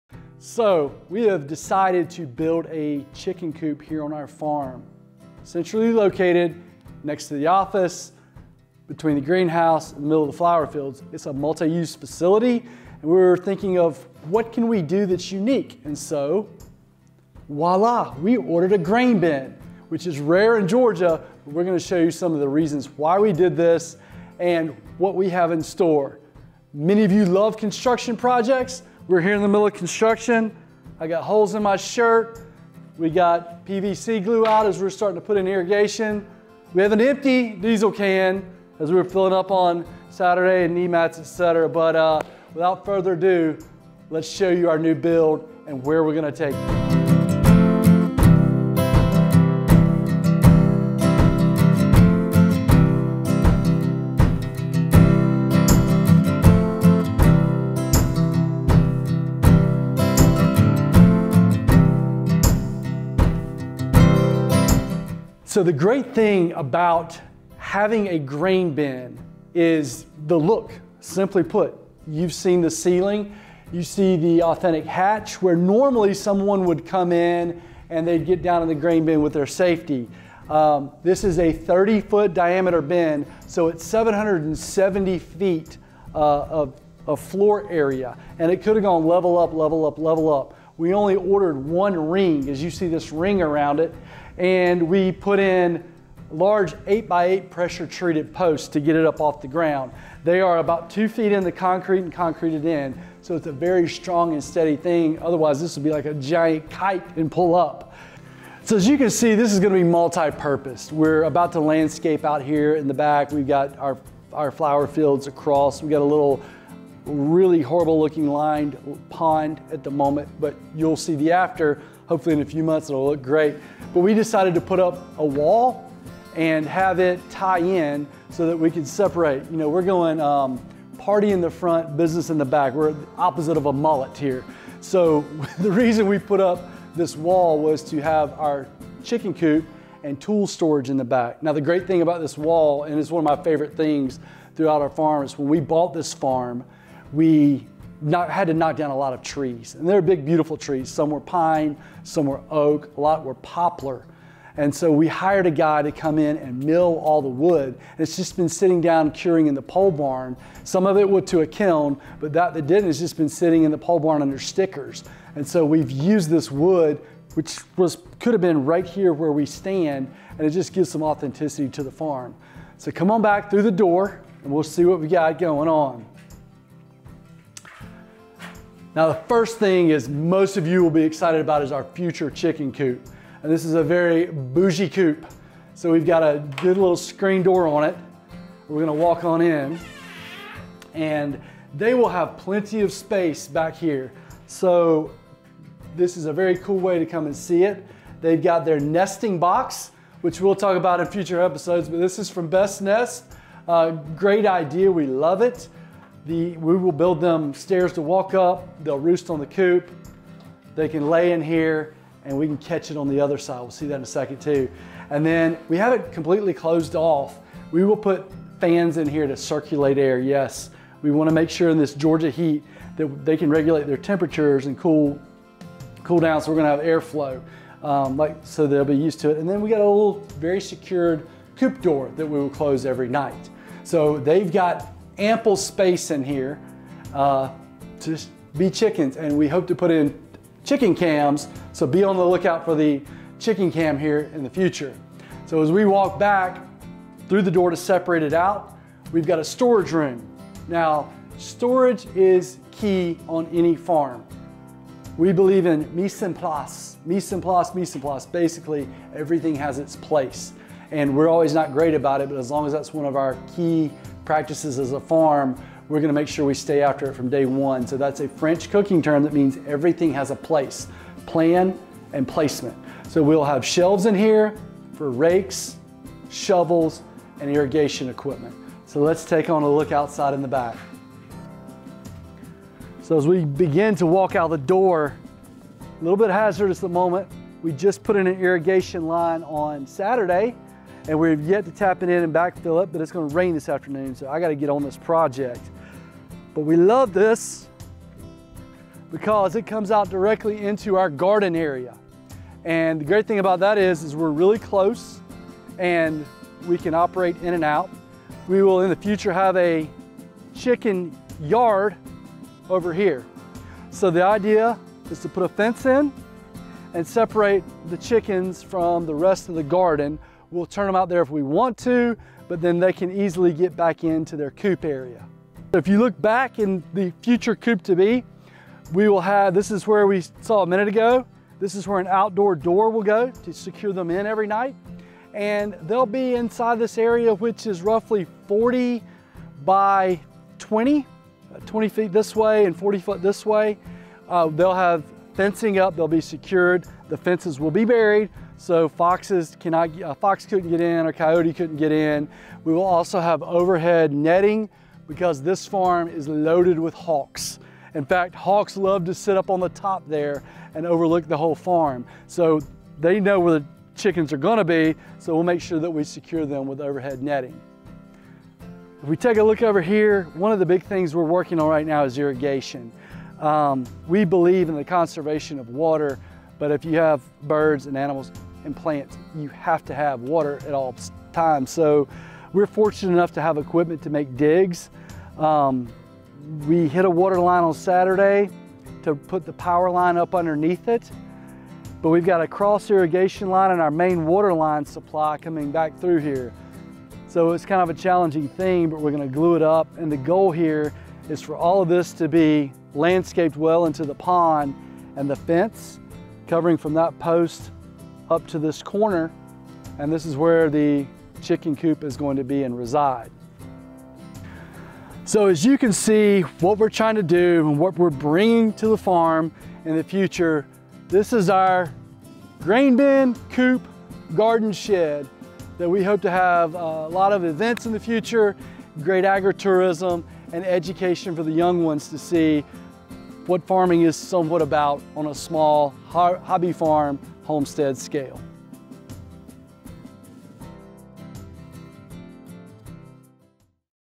Our Grain Bin Chicken Coop Build
We are lucky to have a beautiful slice of land in Bogart, Georgia just outside of Athens. Our small hobby farm includes lavender and eucalyptus fields, a greenhouse with a hydroponics system, flower fields and a garden for our employees. All we've been missing is a chicken coop!

We decided to build our chicken coop as a multi use facility. It's being built between our office and our fields, and adjacent to our garden. Check out this video to get a tour of our progress as of June 2022!

Our farm is on 67 acres of land. Although this is not a huge farm operation, we knew we had the space to build a chicken coop larger than your typical backyard chicken coop! Ours is more of what you might see on a homestead. Plus, it's multi-purpose, so not everything that you see is part of the chicken coop itself.

As far as a chicken coop plan goes, we decided on a grain bin! The main benefit of the grain bin is quite simply that it looks awesome. It's hard to miss since it's so shiny, and it really brings a nice "farm-like" feeling to the space. It fits in well to the landscape and is noticeable without being too distracting or obstructing the view too much.

We bought our grain bin new, but it's more than possible to build a chicken coop out of a repurposed grain bin! It would take nearly the same process as we took to use a repurposed grain bin.

Our grain bin has a 30 foot diameter and 770 feet of floor area. It's large enough to accommodate storage, a porch area, a nesting box and a pretty large chicken coop. We added one ring to the base for height, but theoretically you could add more if you needed more height. We think the one ring is more than sufficient, however.

We love the construction process, and as of this blog post we're still adding details and extra functions to our grain bin chicken coop.

Our grain bin chicken coop is multipurpose. Of course we have the coop for eggs and the fun of raising chickens! But the rest of the grain bin gives us an aesthetically pleasing, convenient means of storage too. We will store tools for our coop as well as equipment for gardening, farming and landscaping here.

The porch area provides a really nice hangout space, too! Here, we can overlook the small pond (which will hopefully look a bit better in the future) while staying close to the garden and office.

Fun fact! When originally building this property, some large trees had to be cut down. We had these trees cut into useable wood- that's what you see forming the wall outside the coop! We were also able to use this wood on the inside of our office.

All in all, the chicken coop portion of our grain bin is quite simple. We made a swinging door with chicken wire that opens from the shed area into the coop. Of course, the walls of the coop are just part of the grain bin.

Inside, we have our nesting boxes from Best Nest. It's pretty high up, so we're going to build little stairs so the hens can climb up and nest. We'll pick eggs out from the nest box from the other side of the coop, in the porch area.

In the coop, there's small square door for the chickens. It leads outside to our chicken run area. This door helps with airflow and allows the chickens to explore outside during the day. We will close the door every night to make surer the birds don't become prey to the other animals that live in this area.

Soon, we plan to start up chicken cam so we can live stream our chicken coop!

As we mentioned, we have a handy storage area just outside of the coop. On our farm, the concept of mise en place is really important to us. This means that everything has a home, a specific place they are stored and can easily be found whenever needed. This is actually a French cooking term for having all of your ingredients prepared and organized before beginning to cook a meal.

The shed will help us maintain our mise en place! We'll have rakes, shovels, irrigation equipment, chicken feed, coop supplies and the like shelved and placed here for convenient use.

We have another door that leads from the shed to our chicken yard. Stepping outside in the video, you can see that we just added irrigation lines around the coop frame. Of course, the chickens won't be out here until this construction and landscaping is finished!

This leads out into a garden area with chicken yard. We are going to construct a fence to separate the chickens from the garden. It will open so they can roam the garden!

The chicken yard area is about 40 by 20 feet. In addition to the fence, we're adding overhead netting so they're protected from the hawks and foxes that frequent our area.

The multipurpose grain bin chicken coop plan is perfect for us, but there are endless chicken coop options for anyone's space and needs.